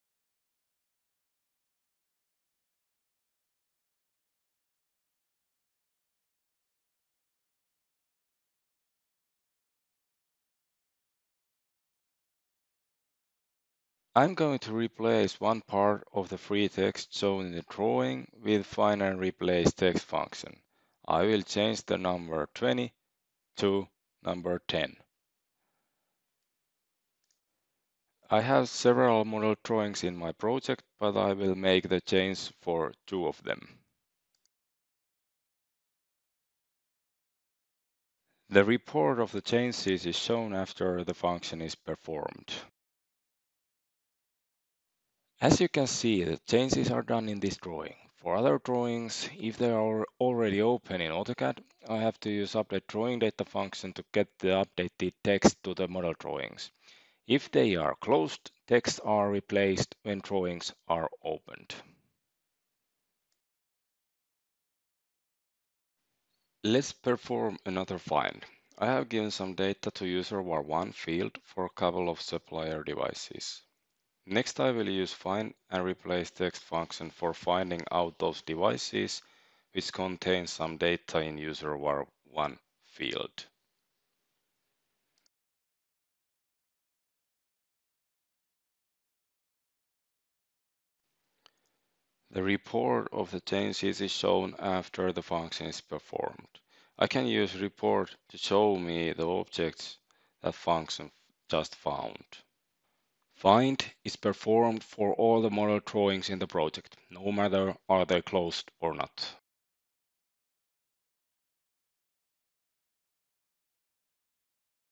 MagiCAD 2016.11 for AutoCAD top new feature - Find and Replace tool for text objects and parameters
MagiCAD for Revit and AutoCAD enables world-leading, data-rich Building Information Modelling for HVAC and electrical design using fast and easy-to-use tools.